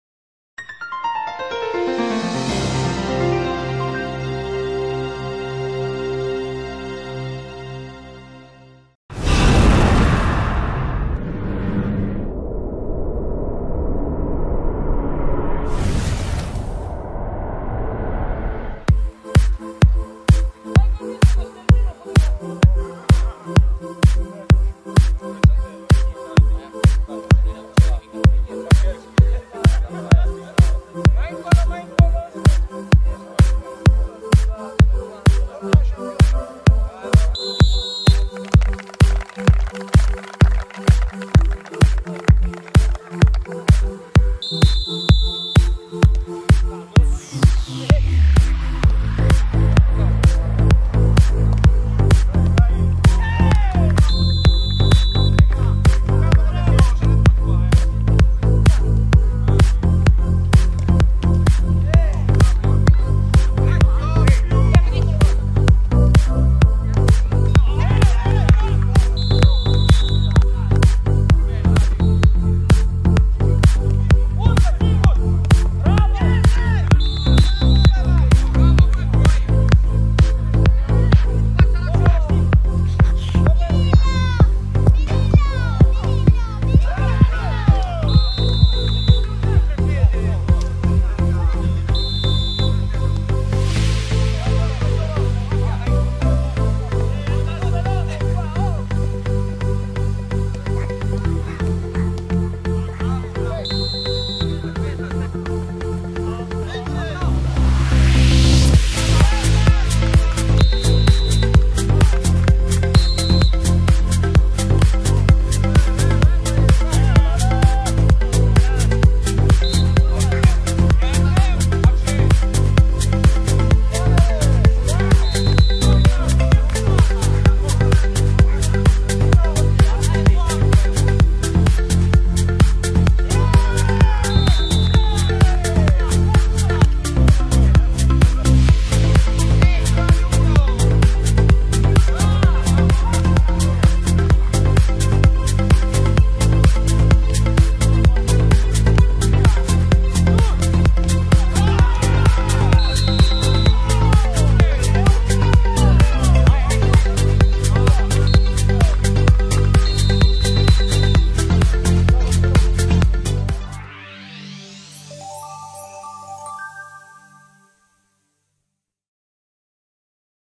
LFAL: La Siepe vs Valgis/Tecno&led - highlights
LFAL Lega Futsal Amatori Latina 1a giornata del campionato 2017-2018 e subito derby al centro sportivo "La Siepe" di Borgo Carso